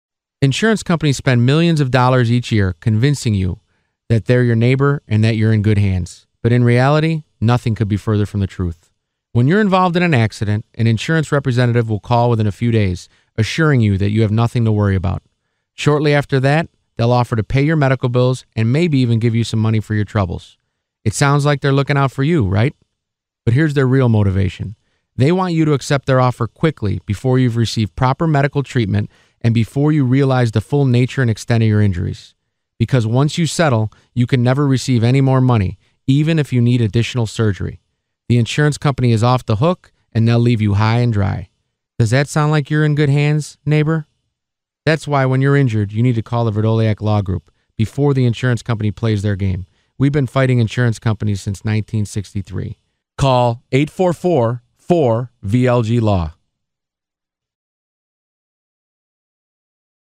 Good Hands | Vrdolyak Law Group
Animated Good Hands Commercial for The Vrdolyak Law Group created by Michael J Webdell LLC